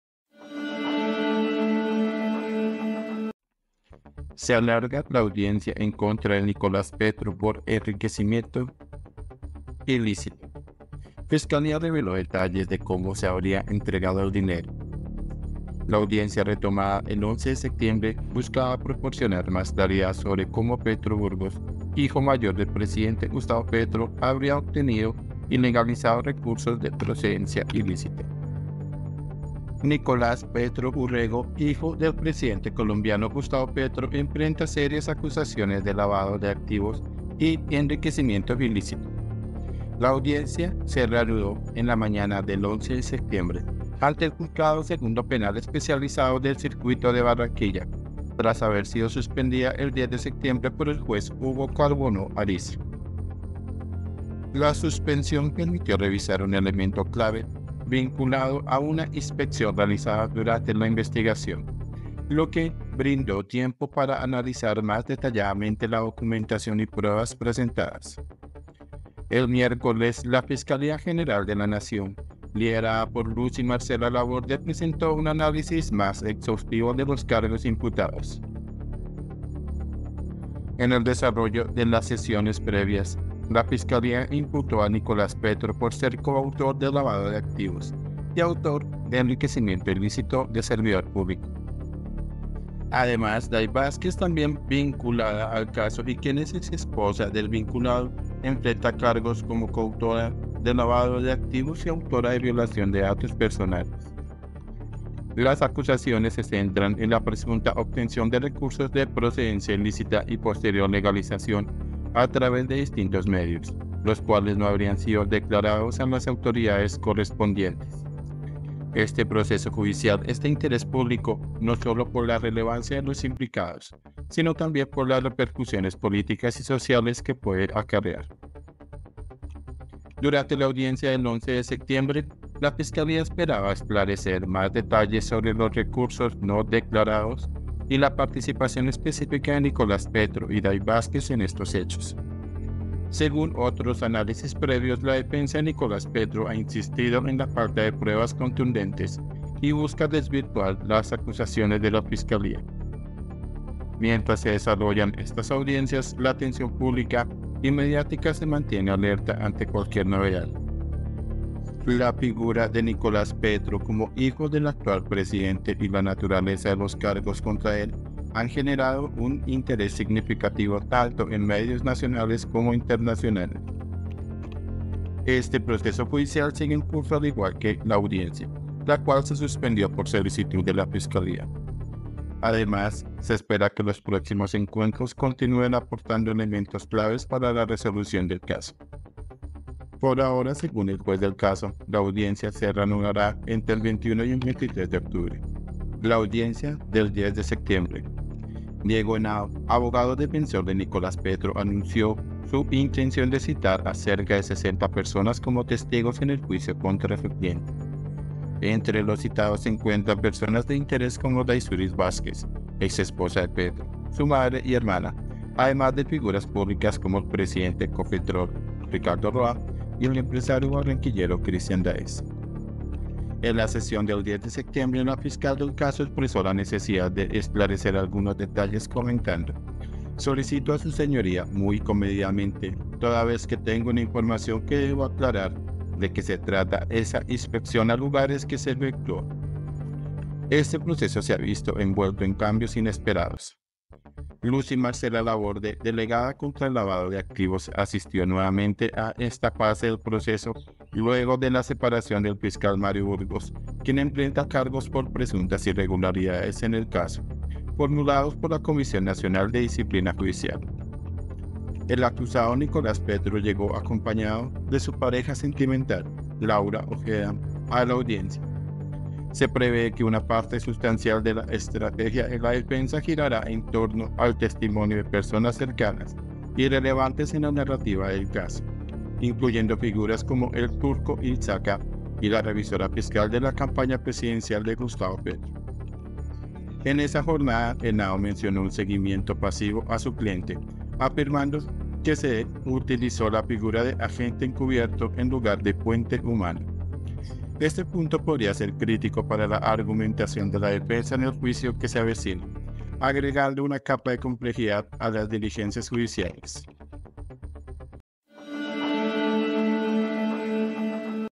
NUEVAS REVELACIONES 📢 Se alarga la audiencia en contra de Nicolás Petro por enriquecimiento ilícito
Se alarga la audiencia en contra de Nicolás Petro por enriquecimiento ilícito: Fiscalía reveló detalles de cómo se habría entregado el dinero
La audiencia retomada el 11 de septiembre buscaba proporcionar más claridad sobre cómo Petro Burgos, hijo mayor del presidente Gustavo Petro, habría obtenido y legalizado recursos de procedencia ilícita
INFOBAE